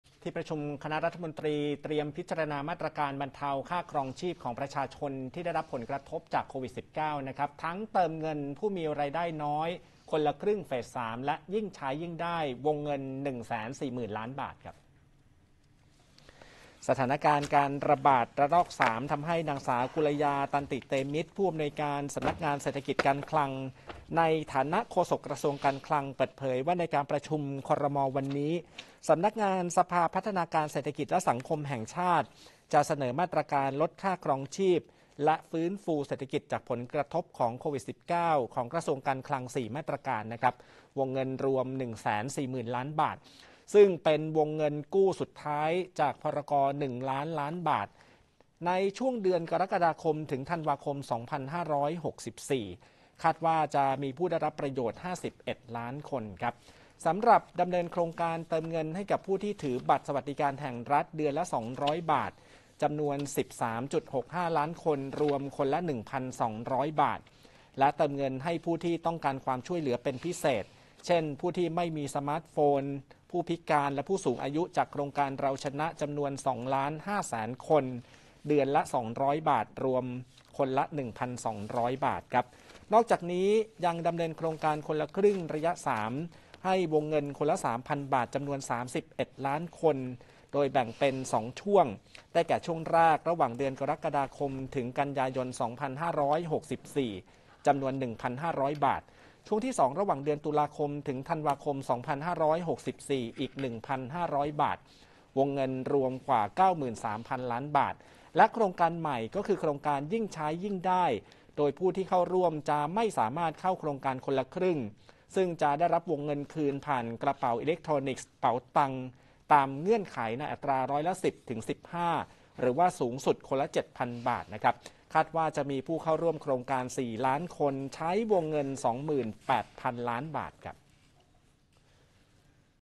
ครม.ถกมาตรการลดค่าครองชีพเพิ่มเติม (1 มิ.ย. 64)
ที่ประชุมคณะรัฐมนตรี เตรียมพิจารณามาตรการ บรรเทาค่าครองชีพประชาชนที่ได้รับผลกระทบจากสถานการณ์โควิด-19 ทั้งเติมเงินผู้มีรายได้น้อย, คนละครึ่งเฟส 3 และยิ่งใช้ยิ่งได้ วงเงิน 140,000 ล้านบาท

ติดตามรายการดีๆของช่อง ได้ที่